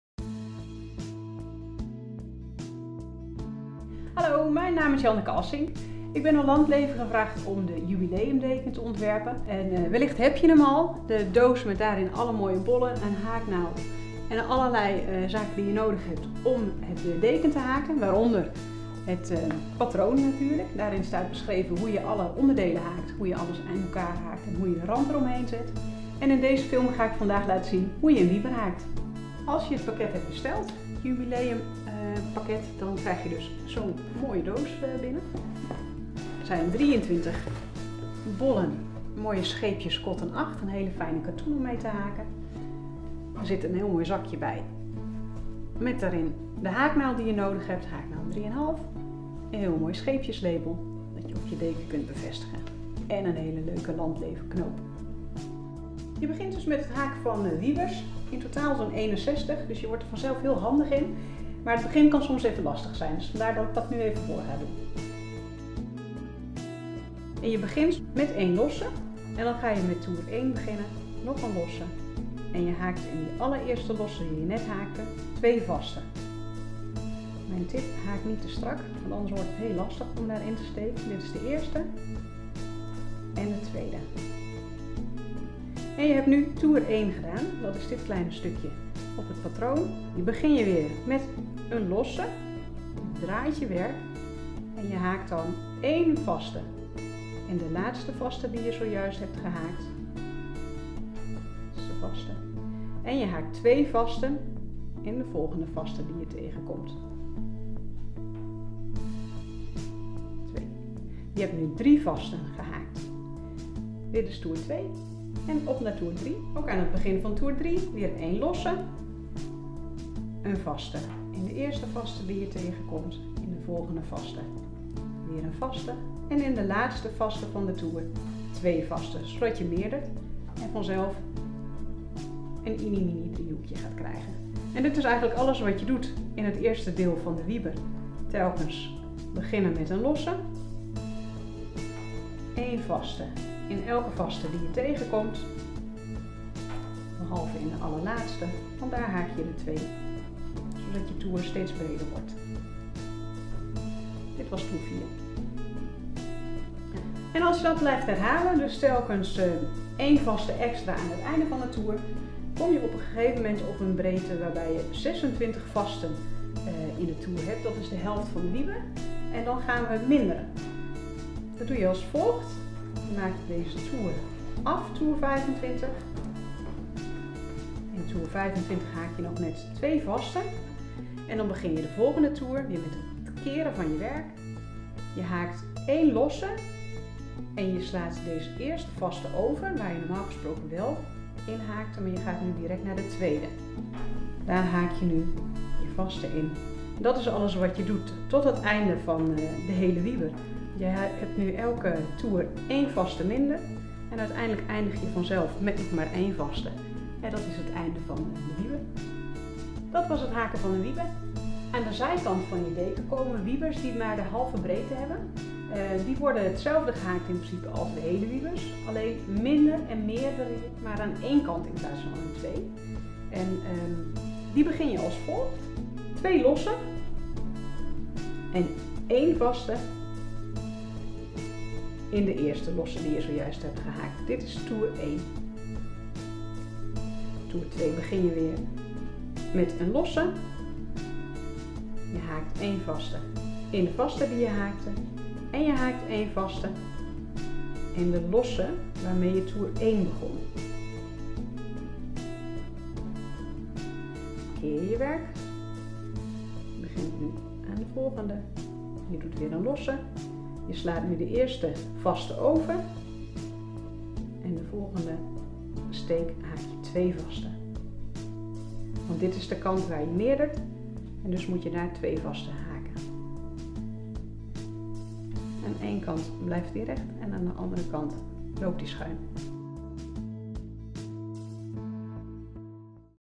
Landleven jubileumdeken haakpakket
Janneke Assink van haakblog Jipbyjan laat zien hoe je de Landleven-jubileumdeken haakt. In het filmpje doet ze voor hoe je een wybertje haakt. Laat je inspireren door Janneke en ga ook aan de slag.